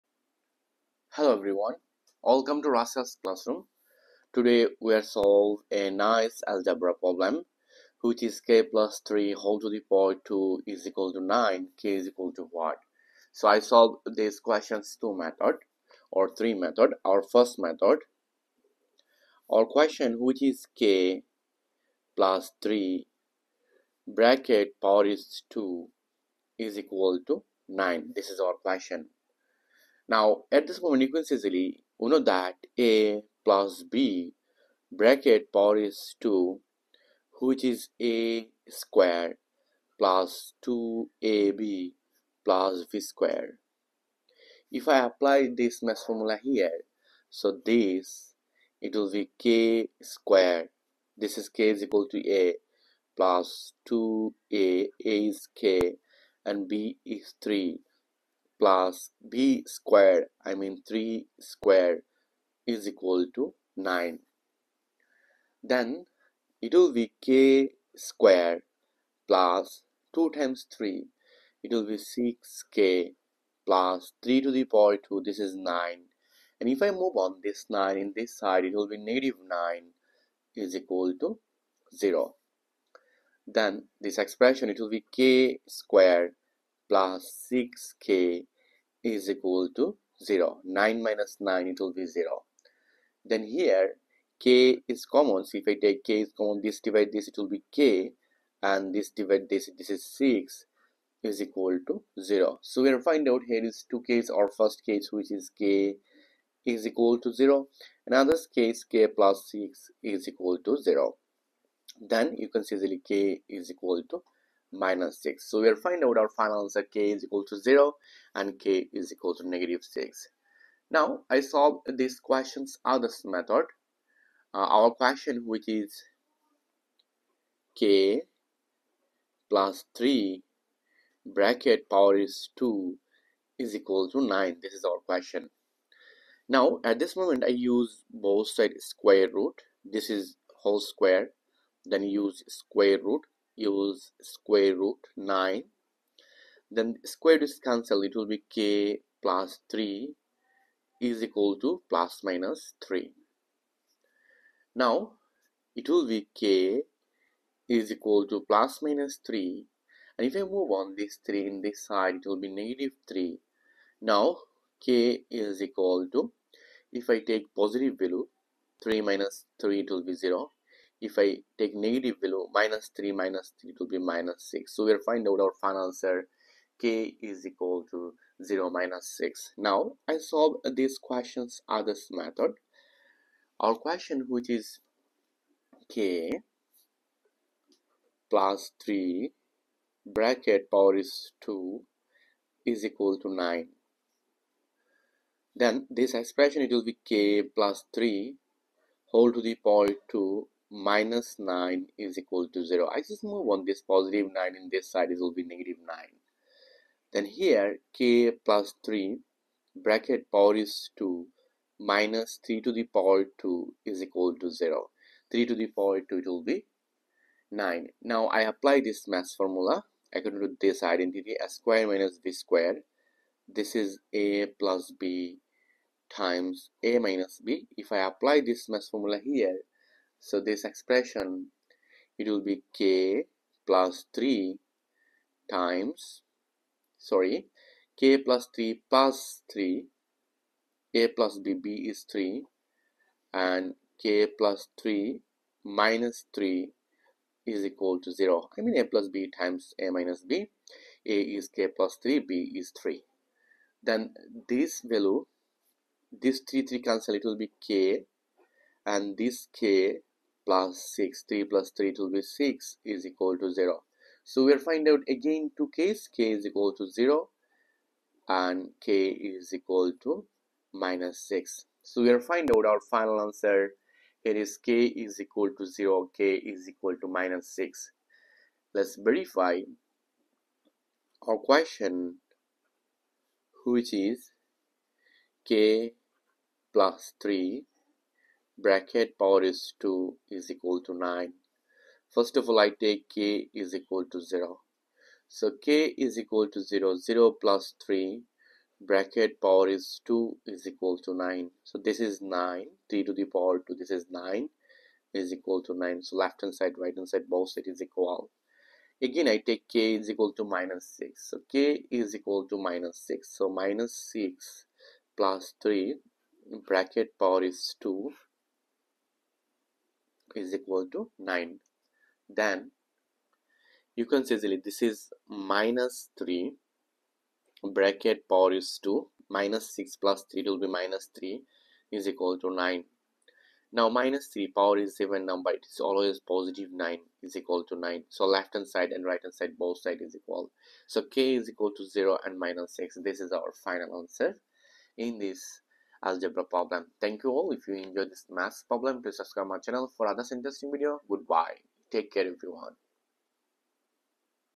A Nice Algebra problem :k=?
Hello everyone ,Welcome to Rashel's classroom. In this video i solve a tricky exponential expression. A nice algebra problem. Find the value of k?